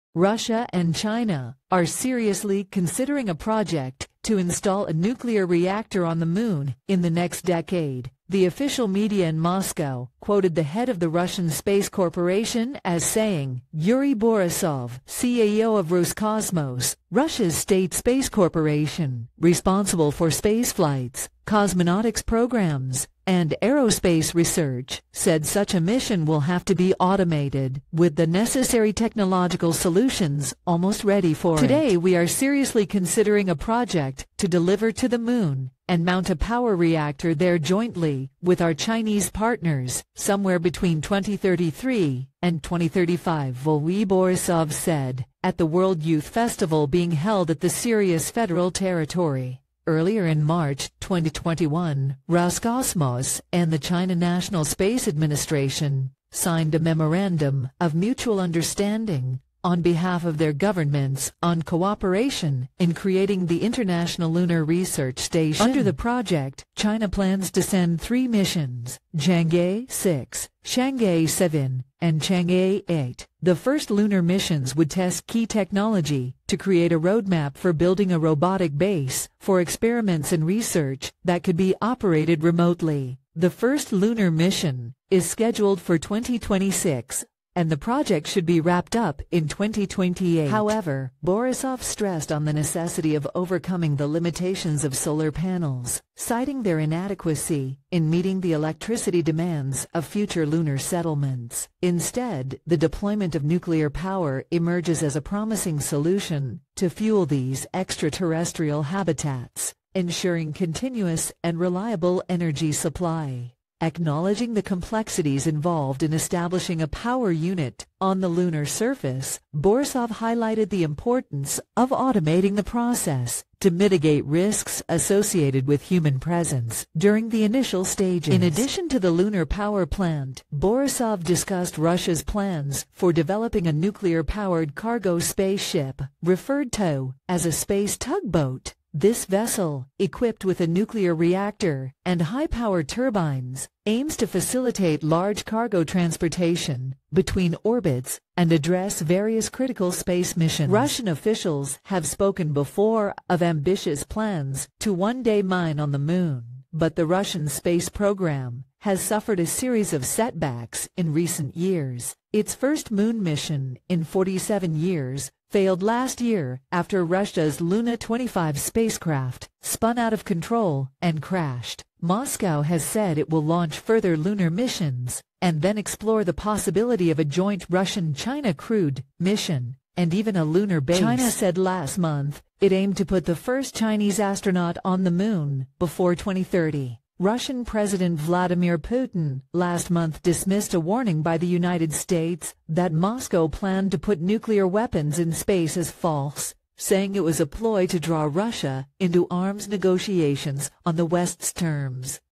Russia and China's Ambitious Goal: Nuclear Reactor on Moon by 2035 - What's Their Decade Plan?
Russian and Chinese officials are reportedly giving serious consideration to a project involving the installation of a nuclear reactor on the Moon within the next decade. This information was shared by the head of the Russian space corporation, as reported by official media outlets in Moscow. For more details, watch the accompanying video.